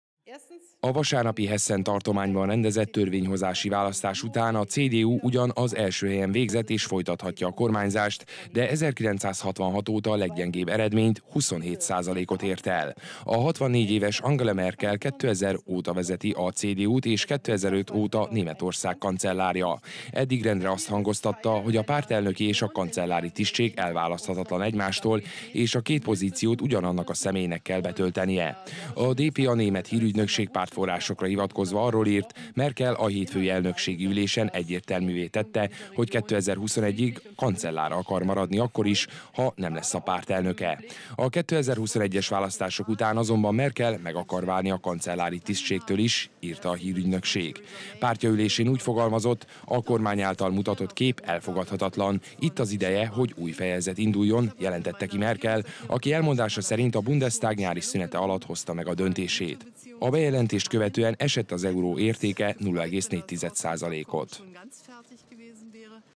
2018.10.29. - Merkel kancellár akar maradni akkor is, ha nem lesz többé a párt elnöke
Lemond Angela Merkel német kancellár a Kereszténydemokrata Unió elnöki tisztségéről a párt decemberi kongresszusán - ezt pártja elnökségi ülésén jelentette be ma Berlinben.

Egyben azt is elmondta: 2021-ben nem indul a kancellári pozícióért. A bejelentés hátterében a tegnapi Hessen-i tartományi választásokon történt gyenge eredmények állhatnak.